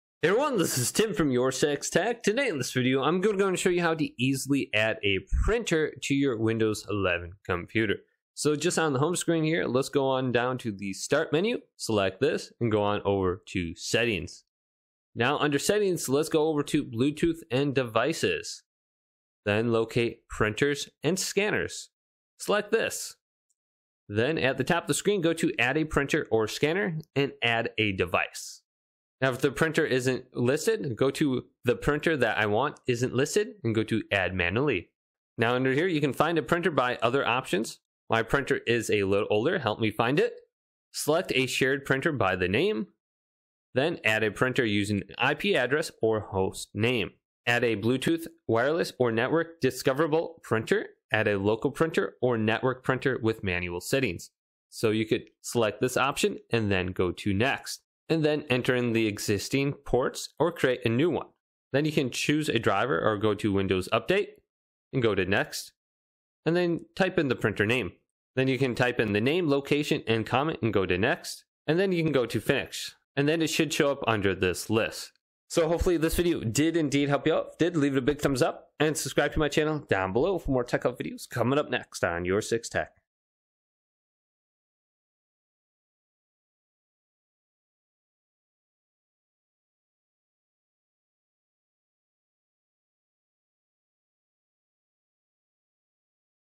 How to add a Local Printer on Windows 11 (Fast Tutorial)
Do you want to know how to add a local printer on windows 11! To do this go to startup and then choose settings, then go to bluetooth and devices. Then choose printers and scanners. Then choose add a device and setup a printer on your windows 10 or 11 PC!